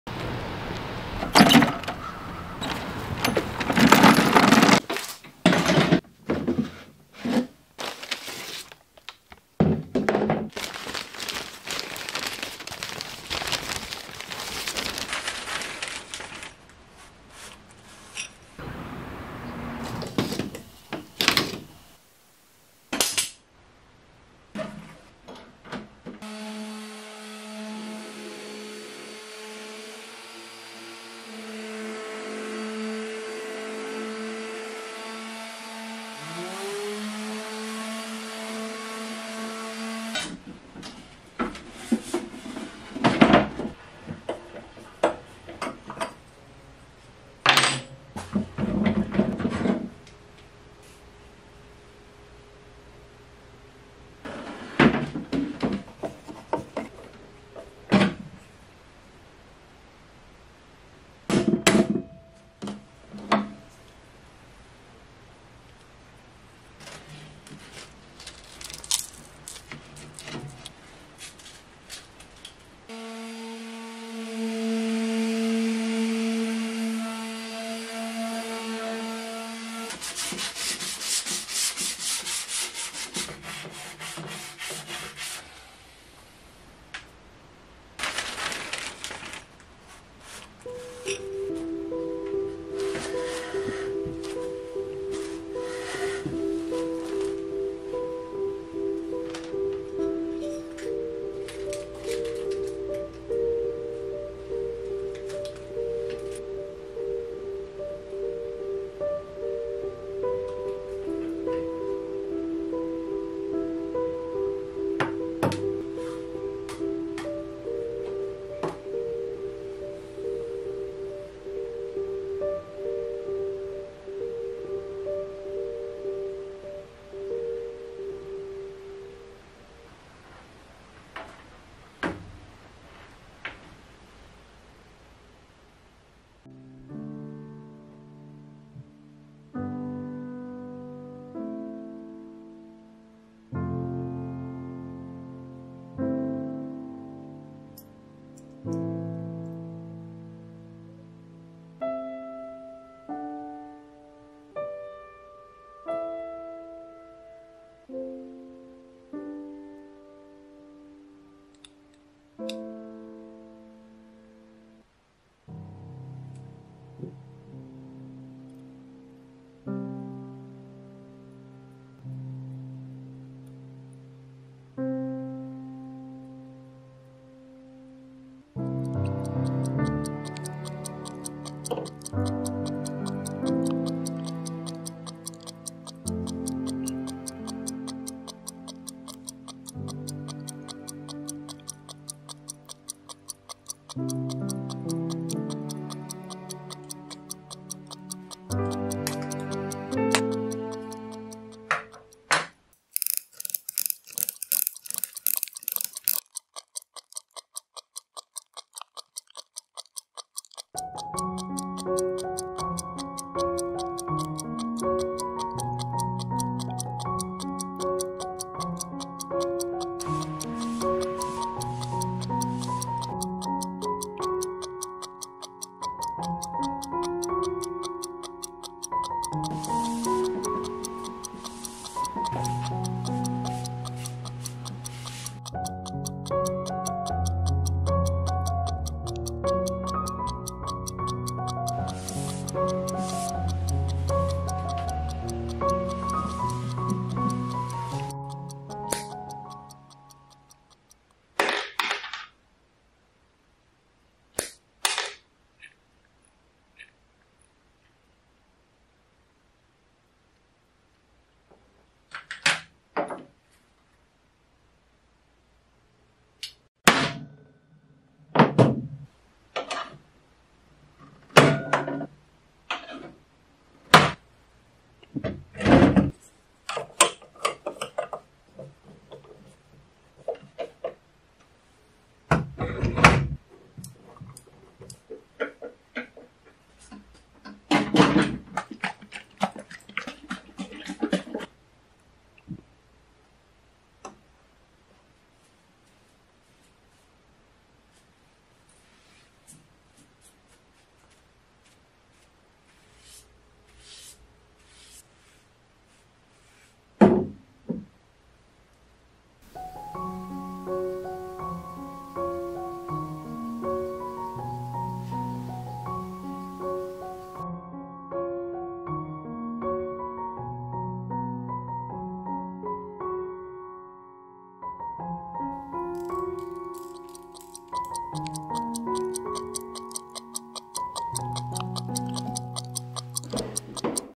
New Life to an Old Table and a Pocket Watch: The Dual Challenge
🛠️ About the Video:
New Life to an Old Table and a Pocket Watch.
This captivating documentary-style video chronicles Mr.Serge's endeavor to restore an old, neglected serving table.  Witness the transformation of not just the table but also the intricate pocket watch, as Serge teaches himself the art of watch repair.

👀  What You'll Discover:
The beauty of bringing a piece of history back to life.
Insights into the world of watch mechanics and woodworking.

Your support means the world to us and fuels our passion for uncovering and sharing unique stories.

We'd love to hear your thoughts! Share your own experiences with restoration, or let us know what you think about Mr.Serge's journey.